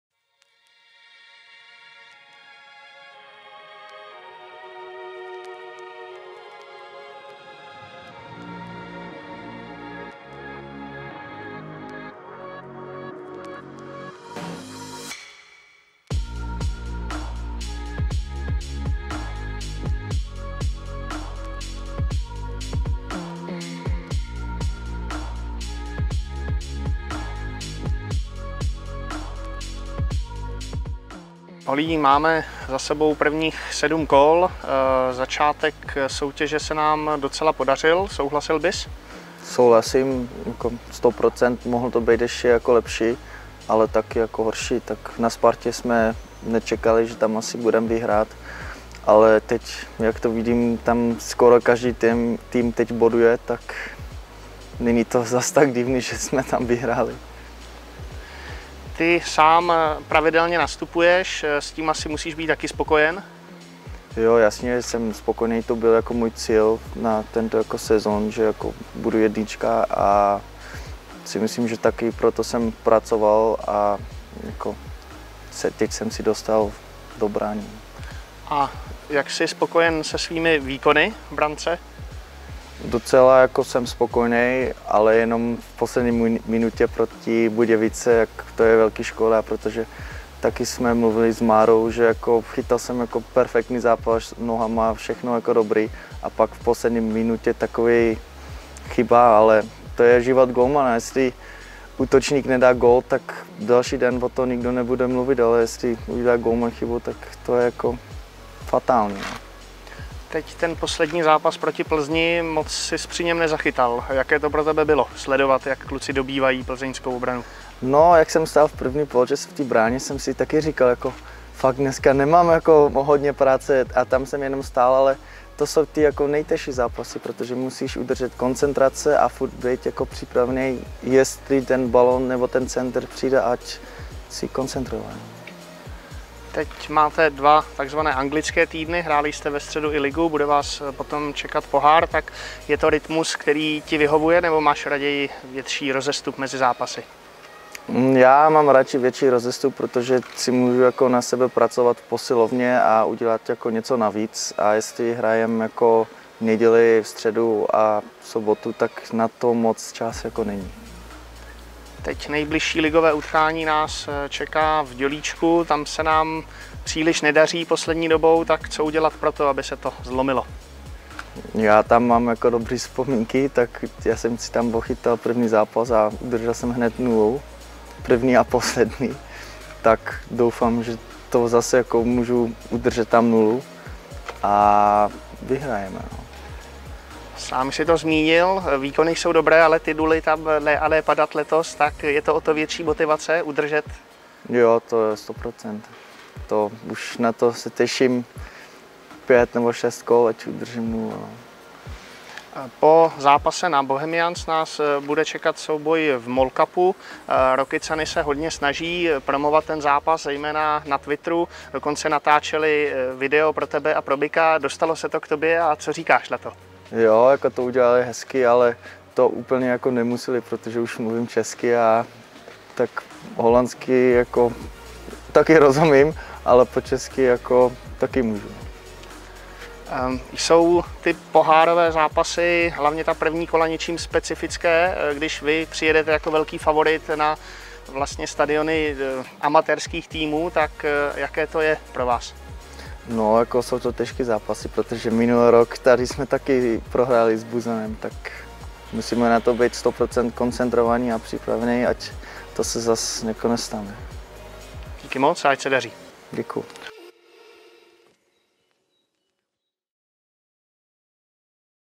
Rozhovor: Olivier Vliegen
Rozhovor s belgickým brankářem Slovanu před zápasy proti Bohemians a Rokycanům doplněný o záběry z tréninku